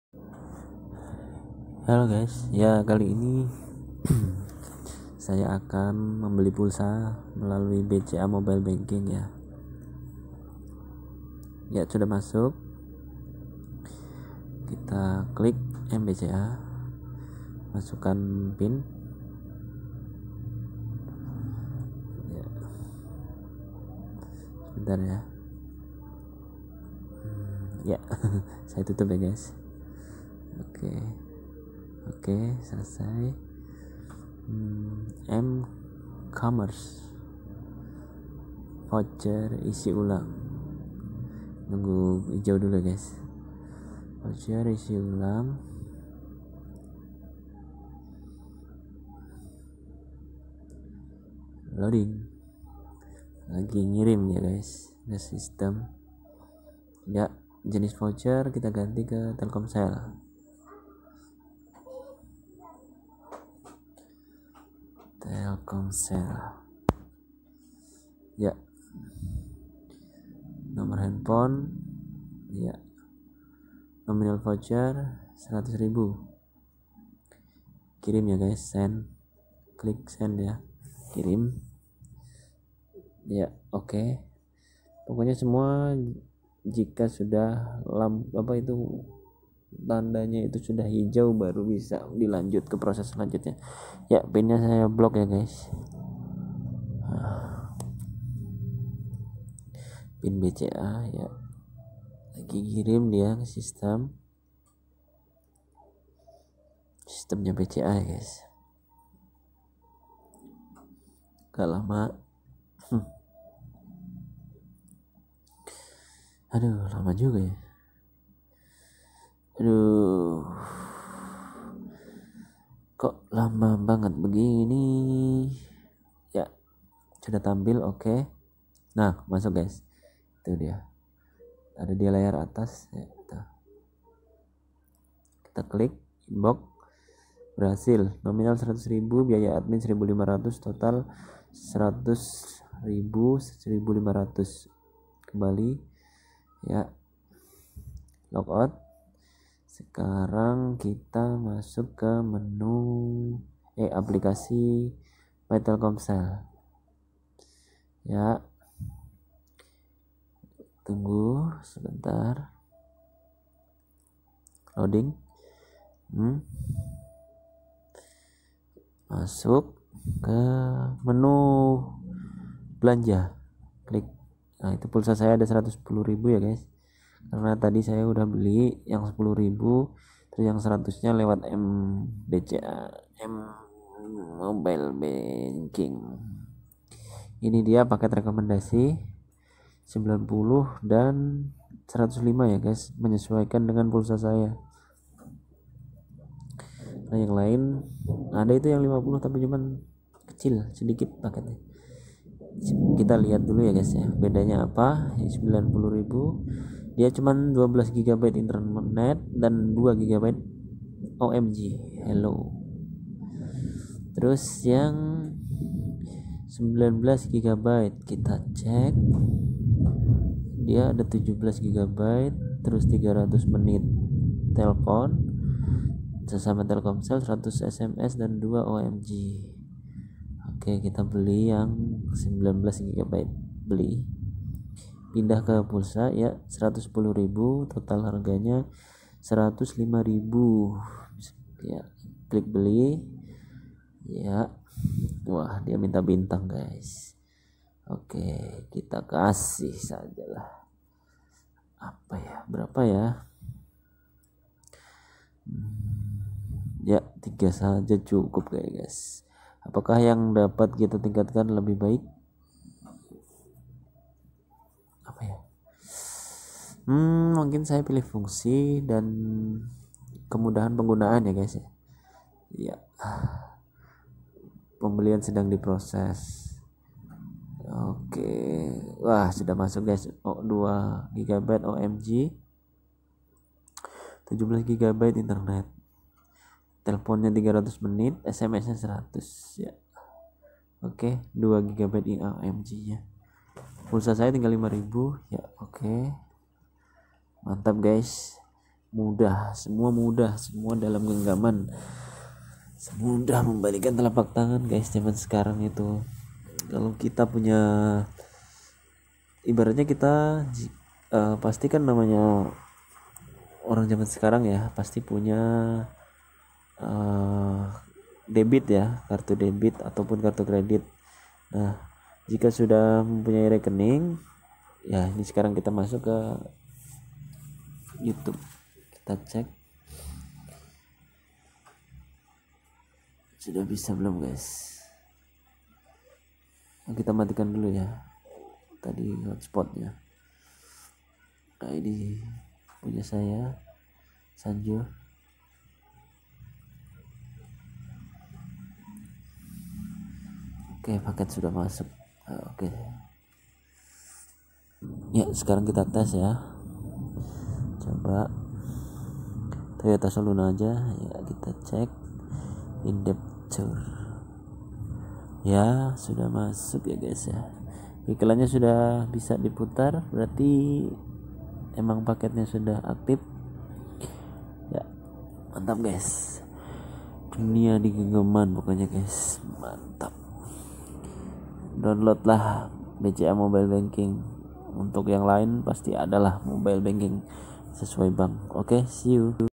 beli pulsa di BCA mobile & beli kuota di my Telkomsel
dengan BCA mobile dunia serasa dalam genggaman kita tidak perlu repot datang ke konter / ke indomart
cukup menggunakan BCA mobile semudah membalikan tangan